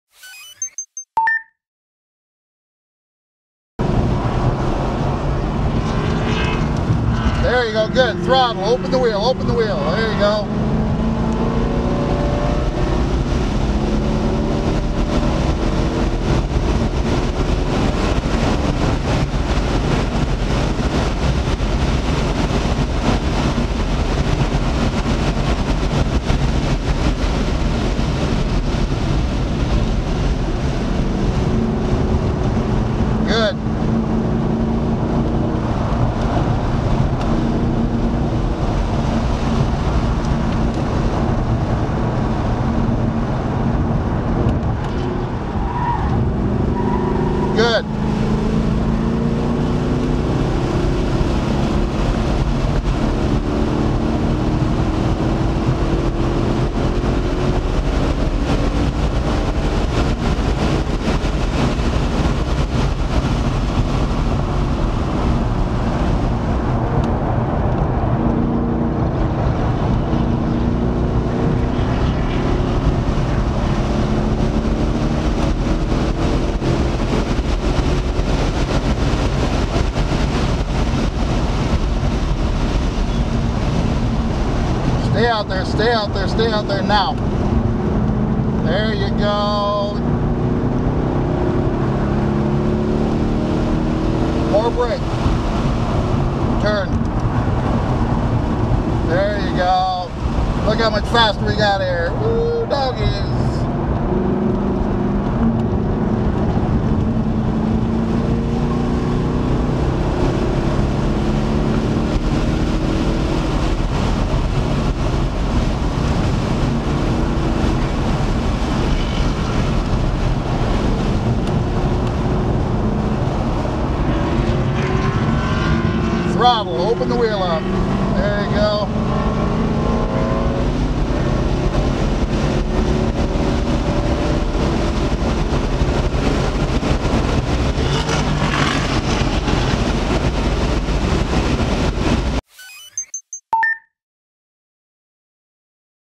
RPM 18-0-602 Brent's Best Lap
Brent's best lap from his first track day.